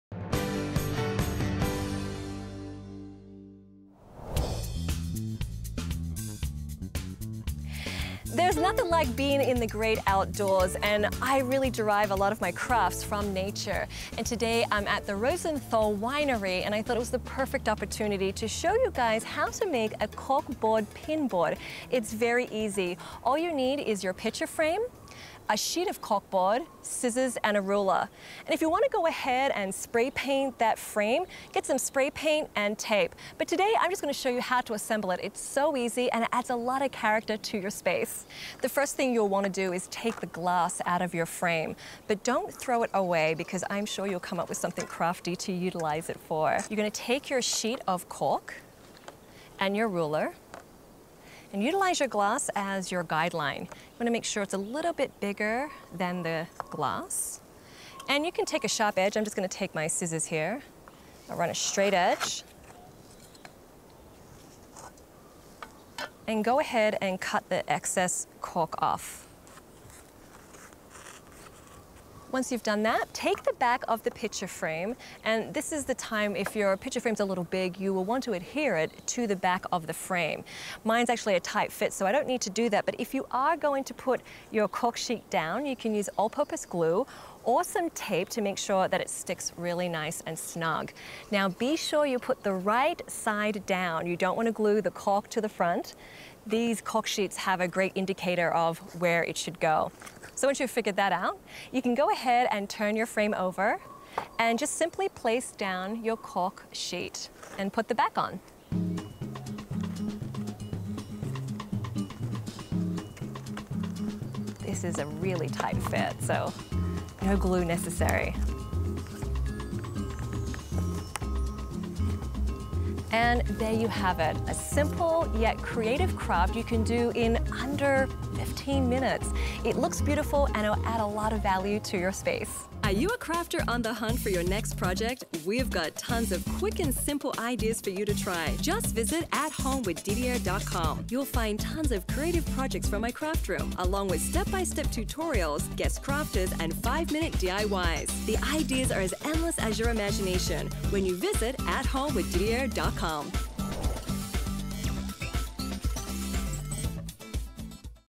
Framed Corkboard Pinboard Craft
In this segment, we learn how to make a framed corkboard pinboard!

Didiayer recommends Fiskars Scissors